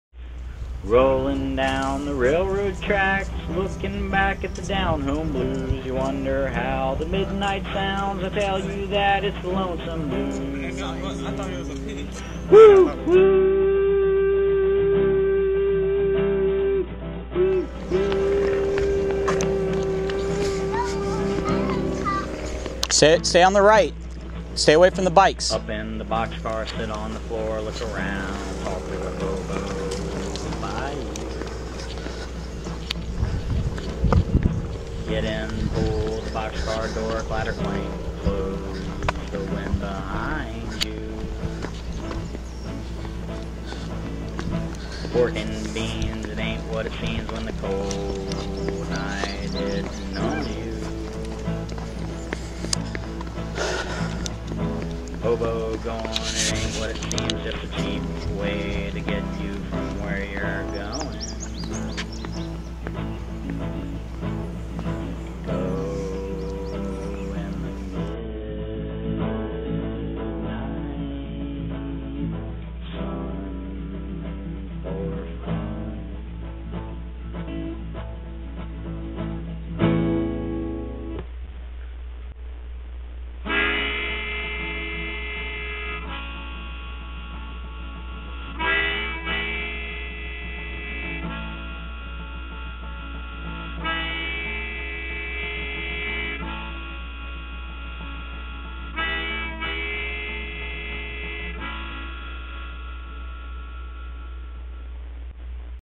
Blue Ridge Subdivision - Greenville Train 2016
Sam Tucker has railroading in his blood. He remembers when hobos took to the rails in boxcars, he remembers when cabooses were the second homes for engineers and he remembers when the Swamp Rabbit moved train cars between Travelers Rest and Greenville.